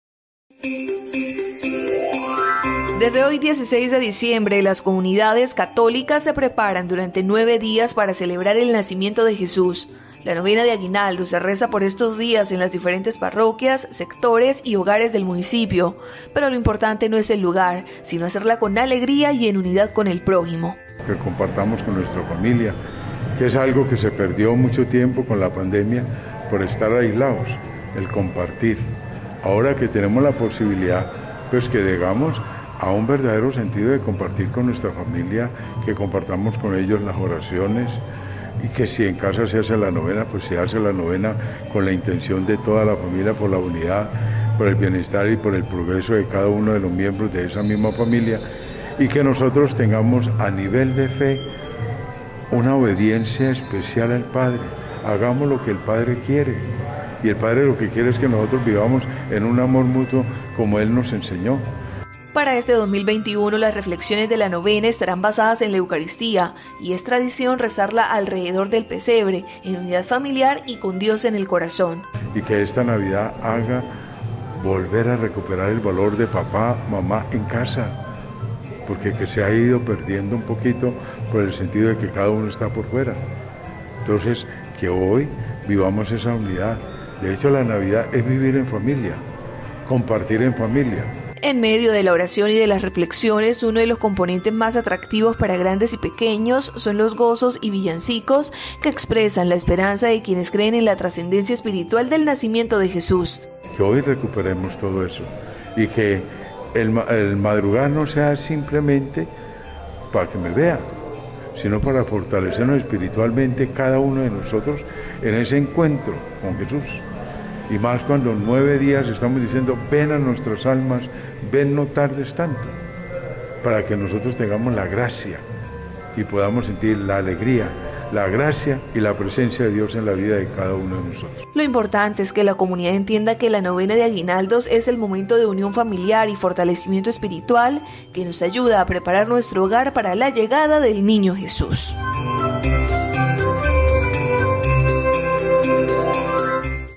Inició la novena de navidad como preparación para el nacimiento del hijo de Dios.
Inició formalmente la novena de navidad y a continuación, Monseñor Luis Carlos Lopera, hace una reflexión sobre lo que significa y la manera como se debe vivir este tiempo de preparación para el nacimiento de Jesús.